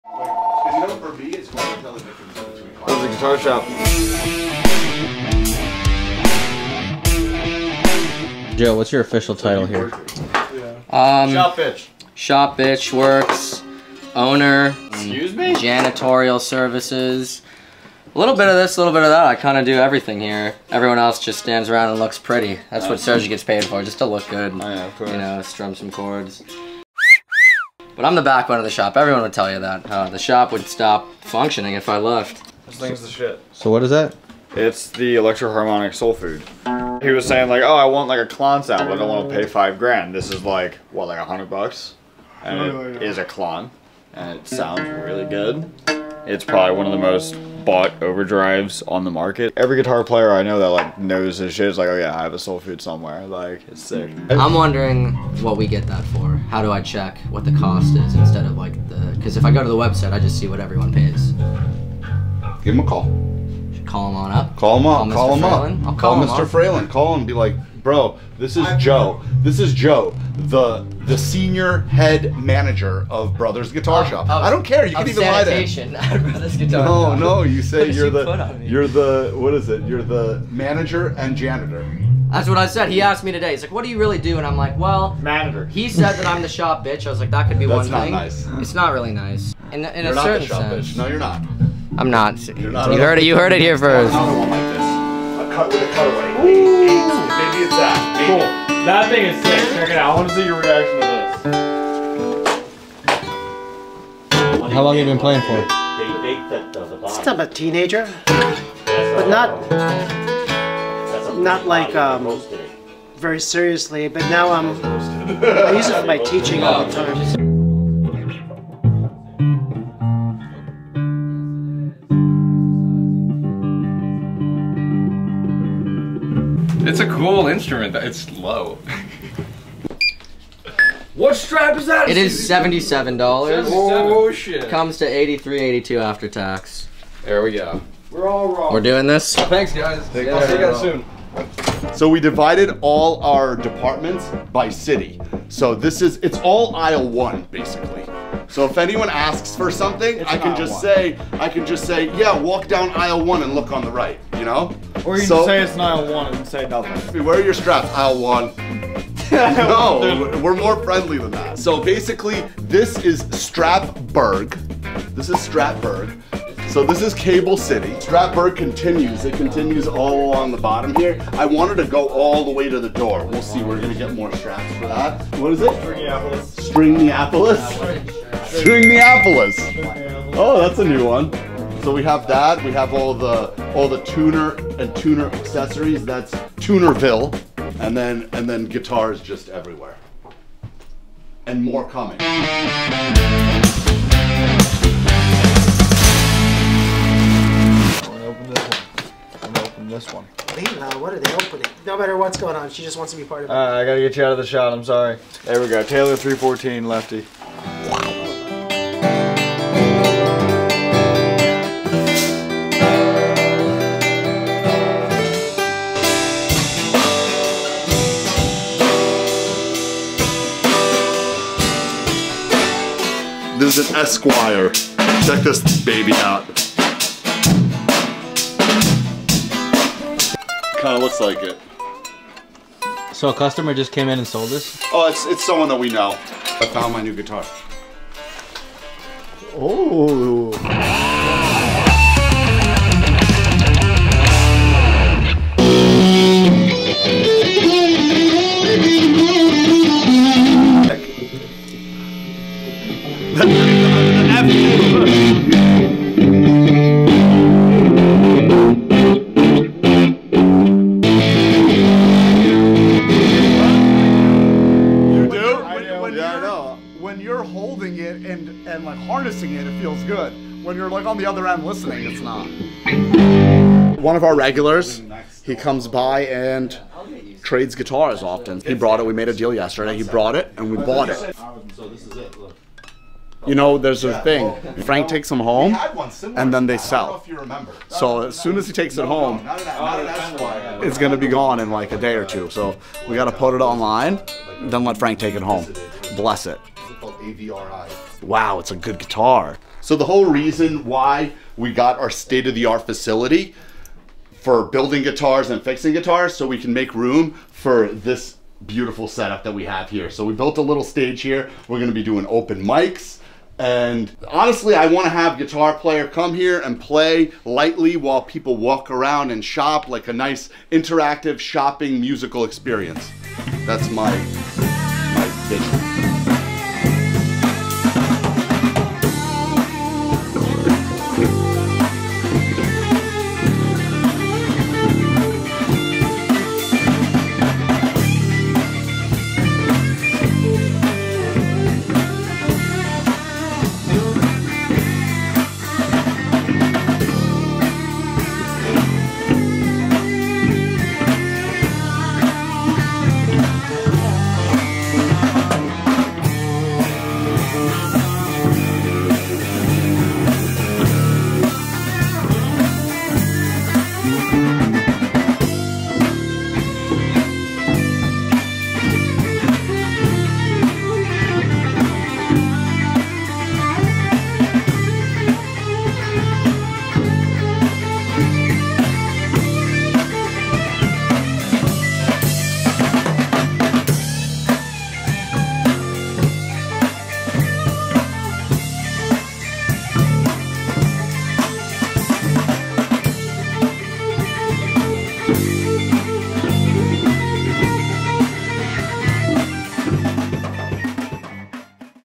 The Crew, The Customers, and The Counties | Brothers Guitar Shop Vlog 3
Meet the Crew and some of our customers.

Get the low down on the EHX Soul food and check out some fender guitars.